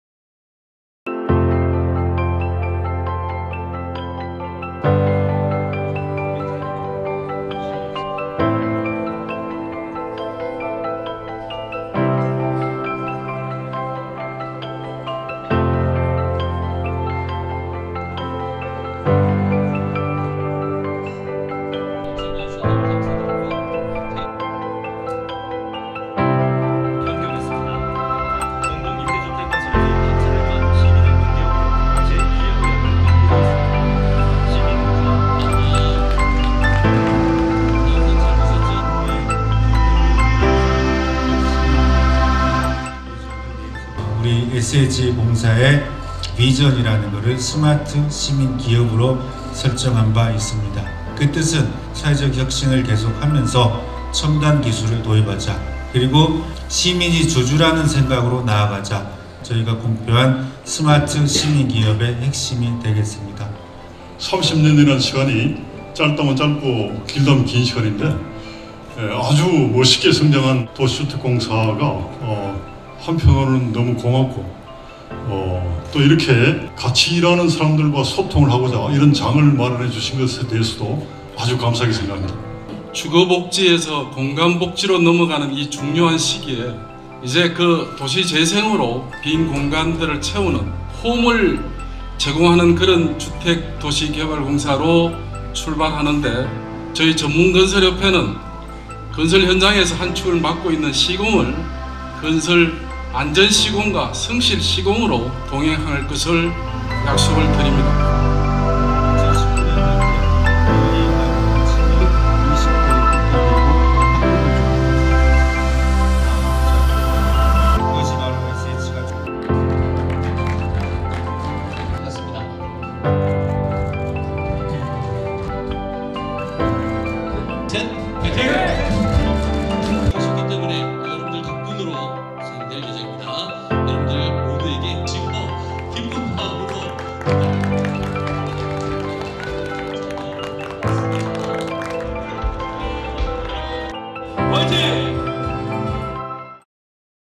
'참 고맙습니다' 서울주택도시공사 파트너스데이 스케치 영상
서울주택도시공사와 함께 일해주어 "참 고맙습니다"
지난 2월 20일 엘타워에서 개최된 '파트너스 데이'!
협력사 임직원 200여 명을 초청해 진행된 이번 행사는 공사 창립 30주년을 맞아 감사의 마음을 전하고자 마련되었는데요!
지금 영상으로 확인해보세요! :-)